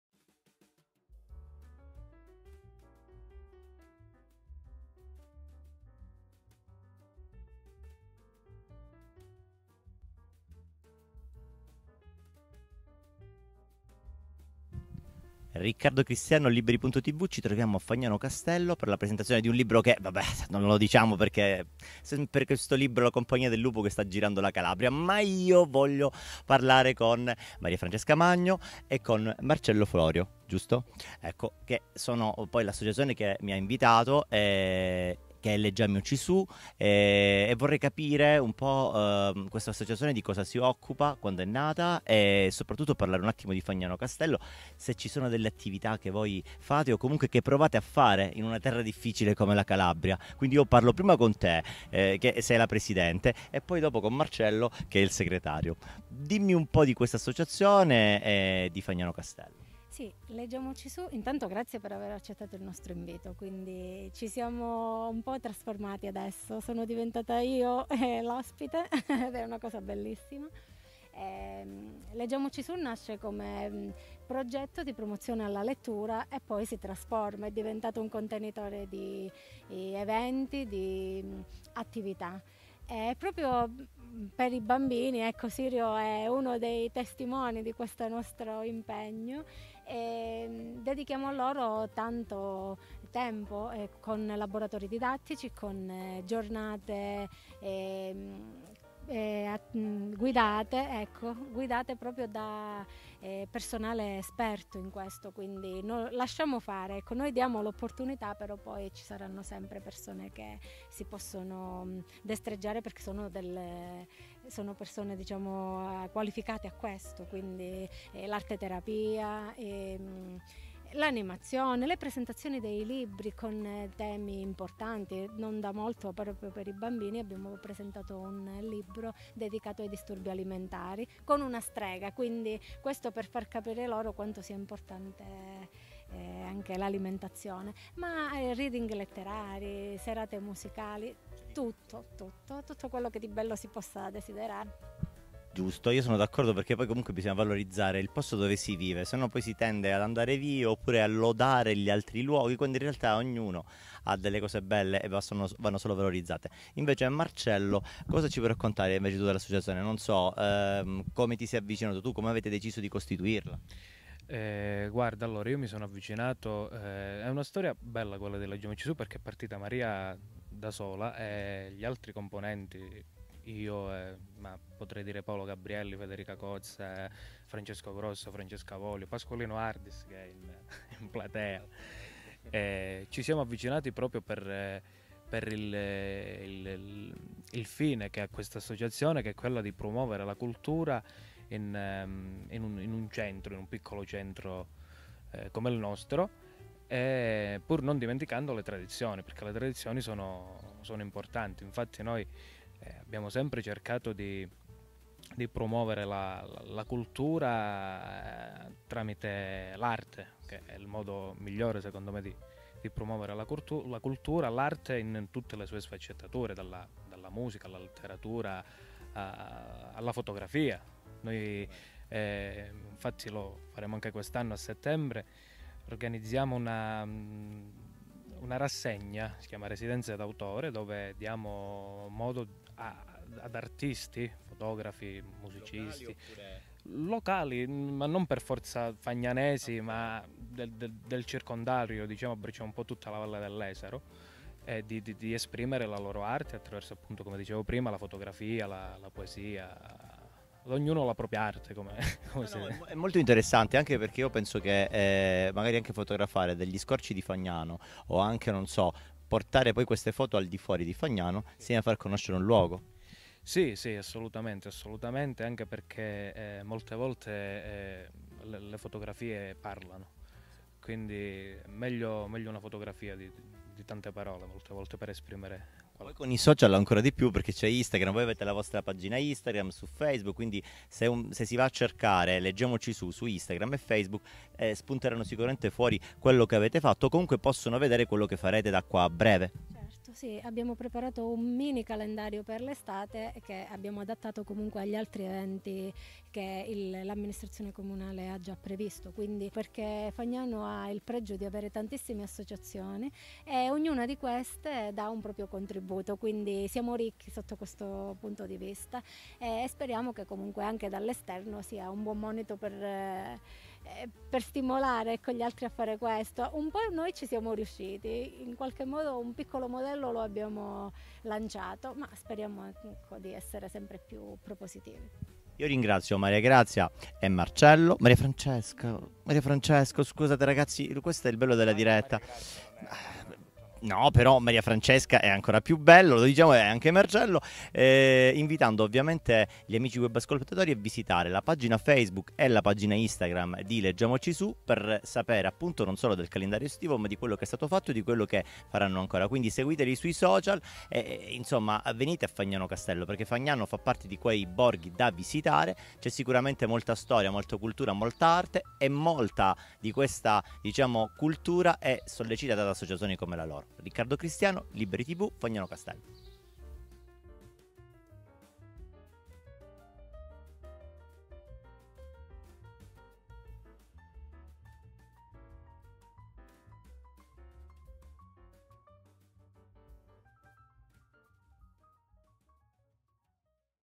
Interviste - Presentazione de "La compagnia del lupo e la quarantesima porta" - Fagnano Castello
Interviste - Presentazione de "La compagnia del lupo e la quarantesima porta" di Riccardo Cristiano -  Ed. Officine Editoriali da Cleto - Fagnano Castello (Cs) - 14 luglio 2022 - A cura di Riccardo Cristiano